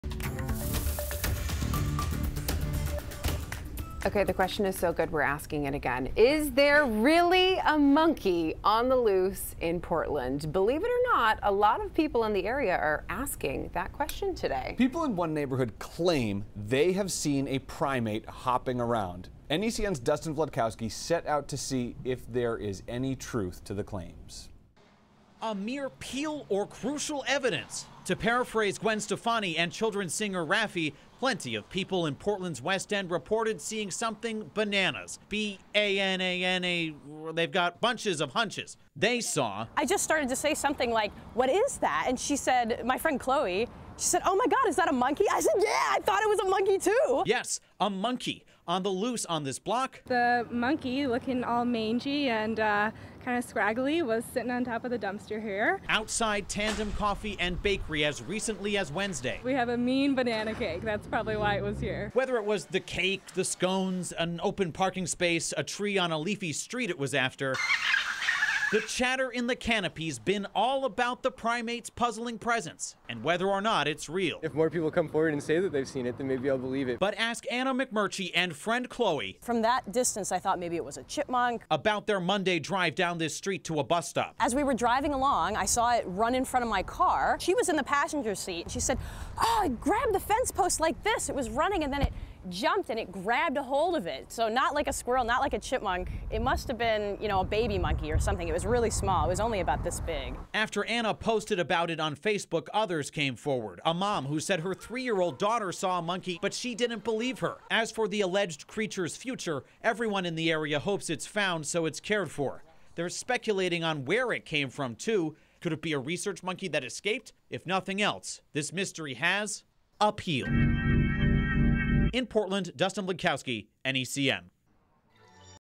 Reports of Mysterious Monkey in Portland
Several witnesses believe they have seen a monkey on the loose in Portland, but the mysterious animal has yet to be captured or claimed.